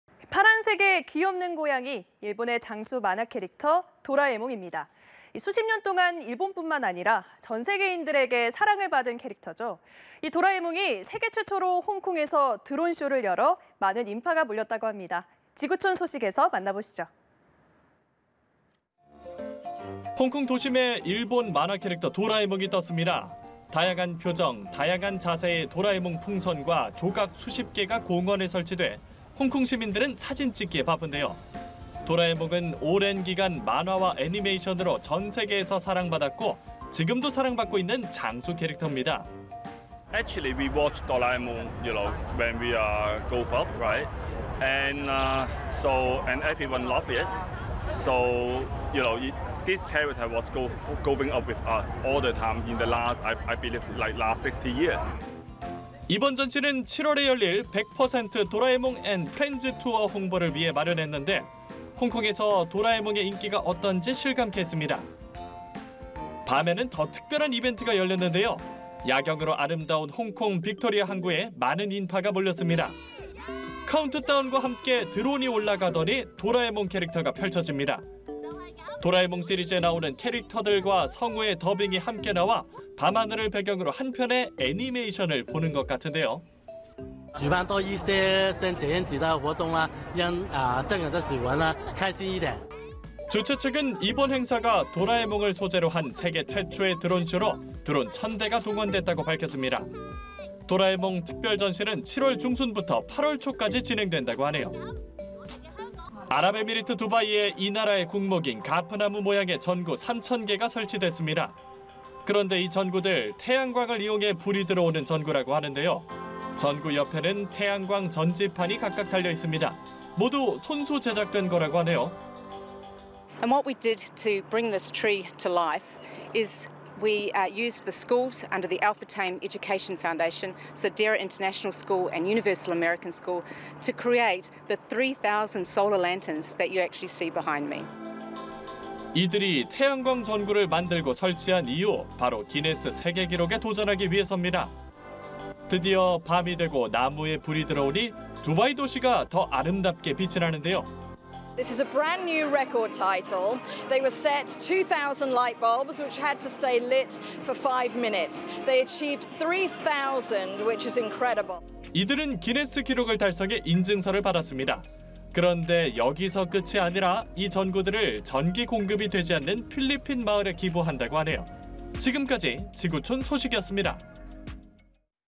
[지구촌톡톡] 일본 장수 캐릭터 '도라에몽', 홍콩 밤하늘을 수놓다 外 / 연합뉴스TV (YonhapnewsTV)
[지구촌톡톡] 일본 장수 캐릭터 '도라에몽', 홍콩 밤하늘을 수놓다 外

파란색의 귀 없는 고양이, 일본의 장수 만화 캐릭터 '도라에몽'입니다.

수십 년 동안 일본뿐 아니라 전 세계인들에게 사랑받은 캐릭터죠.

이 도라에몽이 세계 최초로 홍콩에서 드론쇼를 열어 많은 인파가 몰렸다고 하는데요.

지구촌 소식에서 만나보시죠.

▶ 홍콩 도심에 나타난 일본 만화 캐릭터 '도라에몽'

홍콩 도심에 일본 만화 캐릭터 '도라에몽'이 떴습니다.

다양한 표정, 다양한 자세의 도라에몽 풍선과 조각 수십 개가 공원에 설치돼 홍콩 시민들은 사진 찍기에 바쁩니다.

도라에몽은 오랜 기간 만화와 애니메이션으로 전 세계에서 사랑받았고 지금도 사랑받고 있는 장수 캐릭터인데요.

[에스케이 람(SK Lam) / '100% 도라에몽&프렌즈 투어' 주최자] "사실 우린 자라면서 도라에몽을 봤고, 그렇죠? 그리고 모두가 사랑했습니다. 이 캐릭터는 지난 60년 동안 내내 우리와 함께 성장했습니다."

이번 전시는 7월 열릴 '100% 도라에몽&프렌즈 투어' 홍보를 위해 마련했는데 홍콩에서 도라에몽의 인기가 어떤지 실감케 했습니다.

밤에는 더 특별한 이벤트가 열렸습니다.

야경으로 아름다운 홍콩 빅토리아 항구에 많은 인파가 몰렸는데요.

카운트다운과 함께 드론이 올라가더니 도라에몽 캐릭터가 펼쳐집니다.

도라에몽 시리즈에 나오는 캐릭터들과 성우의 더빙이 함께 나와 밤하늘을 배경으로 한편의 애니메이션을 보는 것 같은데요.

[홍콩 시민] "저도 매우 기쁘고, 아이들도 매우 기뻐하고 있습니다. 이런 행사를 더 많이 열어서 홍콩 사람들이 행복하면 좋겠습니다."

주최 측은 이번 행사가 도라에몽을 소재로 한 세계 최초의 드론쇼로, 드론 1천대가 동원됐다고 밝혔습니다.

도라에몽 특별전시는 7월 중순부터 8월 초까지 진행된다고 하네요.

▶ 손수 만든 3천 개 '태양광 전구'로 나무 모양 전시

아랍에미리트 두바이에 이 나라의 국목인 '가프 나무' 모양의 전구 3천개가 설치됐습니다.

그런데 이 전구들은 태양광을 이용해 불이 들어오는 전구라고 하는데요.

전구 옆에는 태양광 전지판이 각각 달려있습니다.

모두 손수 제작된 거라고 하네요.

[케리 로스(KERRY ROSS) /  '알 푸타임' 교육재단 최고운영책임자] "이 나무에 생명을 불어넣기 위해 우리가 한 일은 알 푸타임 교육 재단 산하의 학교(데이라 국제학교, 유니버설 아메리칸 학교)가 제 뒤에 보이는 3천 개의 태양광 전구를 만들었습니다."

이들이 태양광 전구를 만들고 설치한 이유, 바로 기네스 세계기록에 도전하기 위해서입니다.

드디어 밤이 되고 나무에 불이 들어오니 두바이 도시가 더 아름답게 빛이 나는데요.

[에마 브레인(EMMA BRAIN) / 기네스 세계기록 공식 심사위원] "이건 새로운 기록 부문입니다. 그것은 '2천개 전구가 5분간 불 켜진 상태를 유지할 것'이었는데요, 3천개 목표를 달성했네요. 믿을 수 없습니다."

이들은 기네스 기록을 달성해 인증서를 받았습니다.

그런데 여기서 끝이 아니라 이 전구들을 전기 공급이 되지 않는 필리핀 마을에 기부한다고 하네요.

지금까지 지구촌 소식이었습니다.